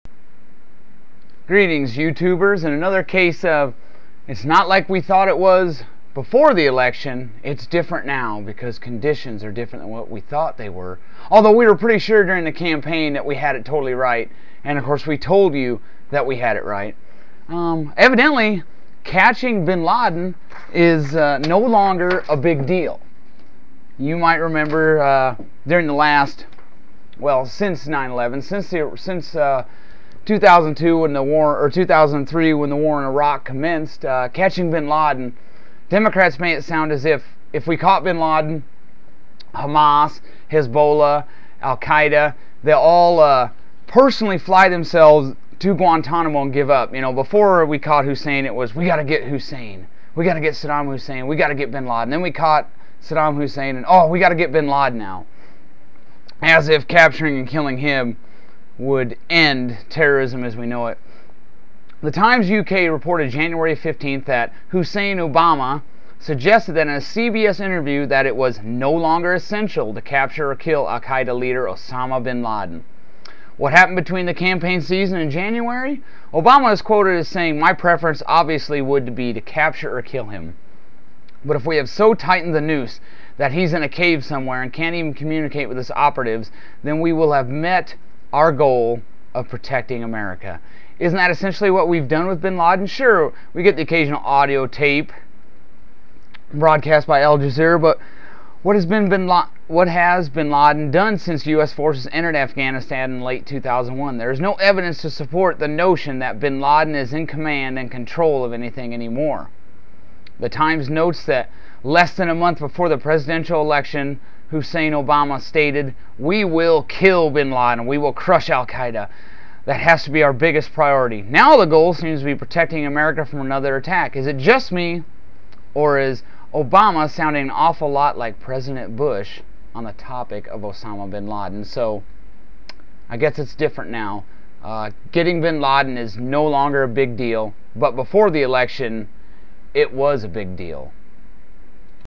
Capturing Osama Bin Laden no longer a big deal?
Hussein Obama and Democrats en masse have been telling us that capturing Osama Bin Laden was of the utmost importance. They implied that catching Osama Bin Laden would basiclaly end terrorism as we know it and placed the utmost emphasis on it. It seems that has "changed" now. That was before the election, but Hussein Obama's rhetoric changed not long thereafter. I wonder what else will "change" in the coming weeks?